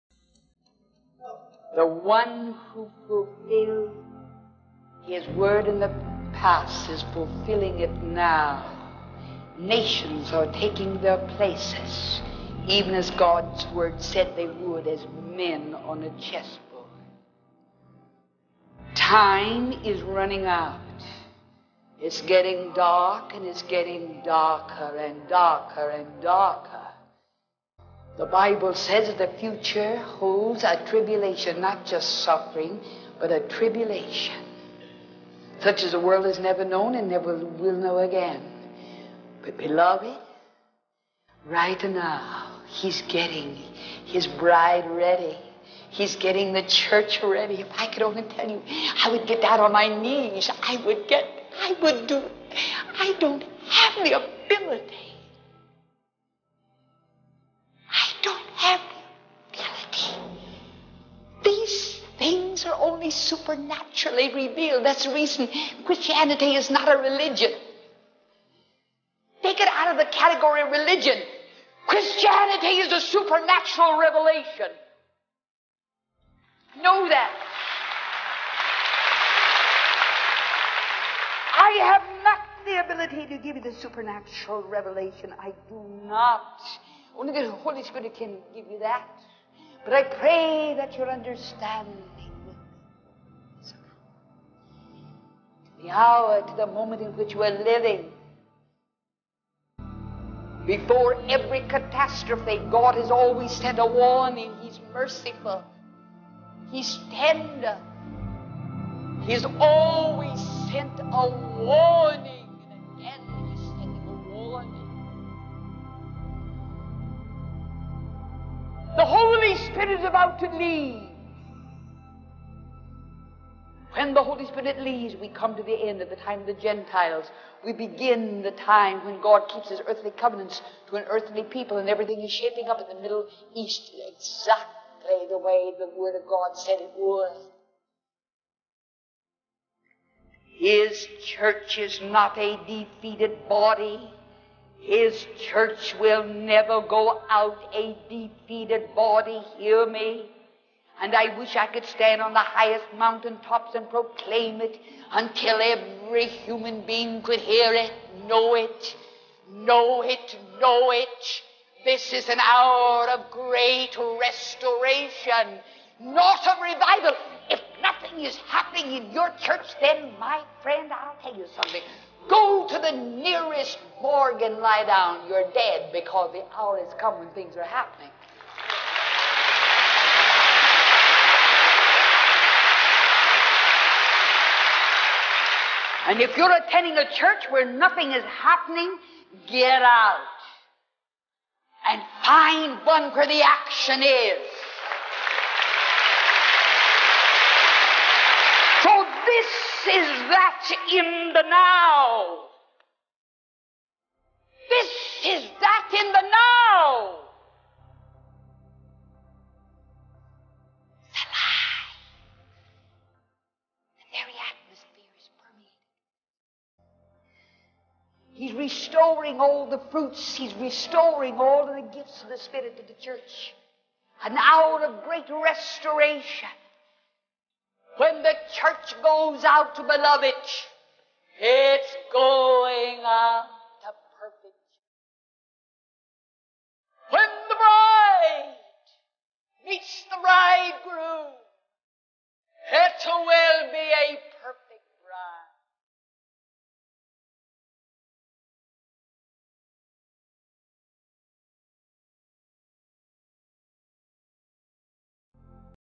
Kathryn Kuhlman || Christianity is not a RELIGION it is a supernatural REVELATION
Christianity is not a RELIGION, Christianity is a supernatural REVELATION